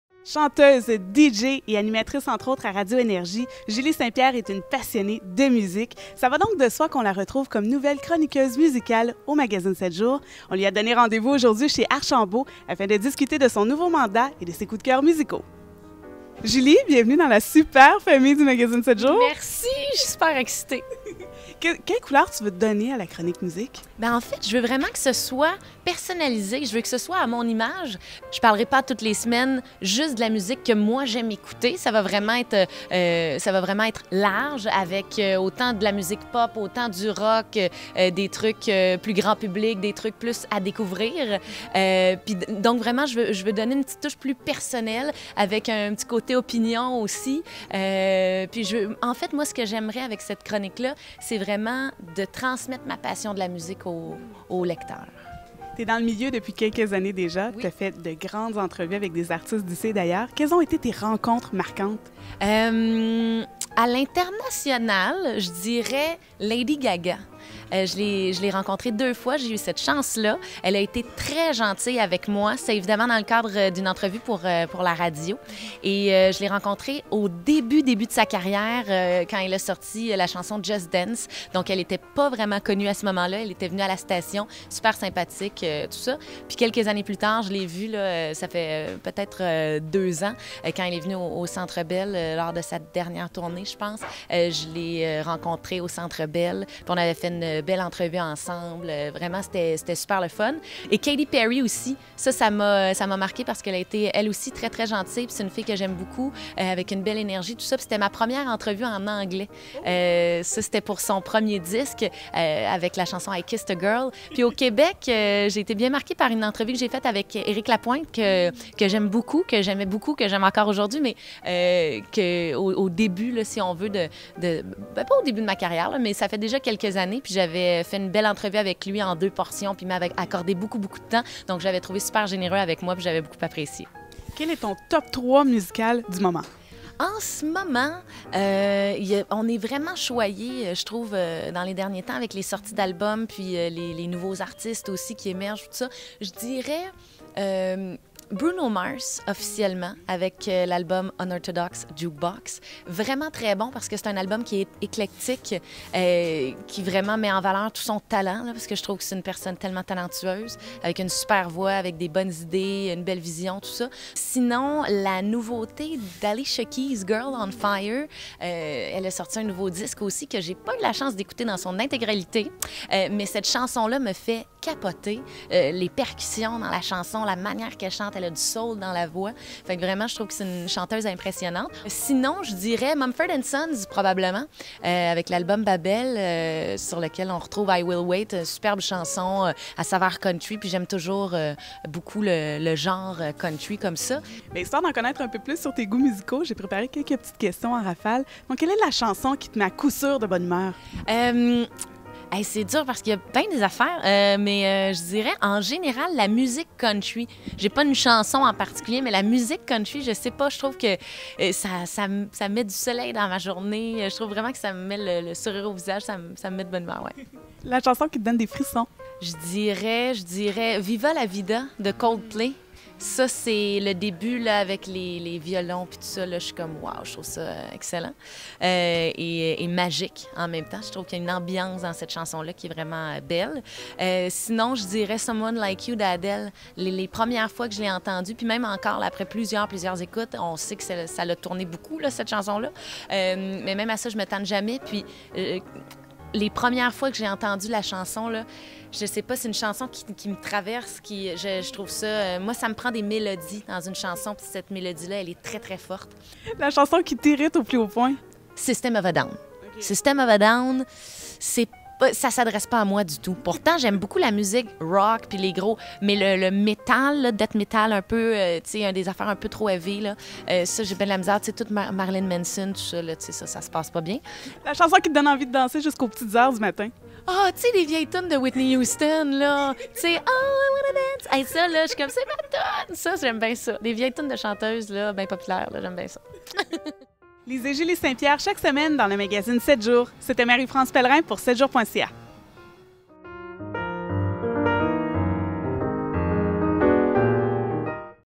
Julie St-Pierre: la musique dans la peau!
La musique nous fait danser, pleurer, sourire et nous irrite même, parfois! Rencontrée chez Archambault, Julie St-Pierre, nouvelle chroniqueuse musicale du magazine 7 Jours, nous fait découvrir ses coups de cœur en tous genres. L'animatrice, qui a rencontré plusieurs grandes vedettes d'ici et d'ailleurs au fil des ans, en profite également pour nous parler de ses entrevues les plus mémorables.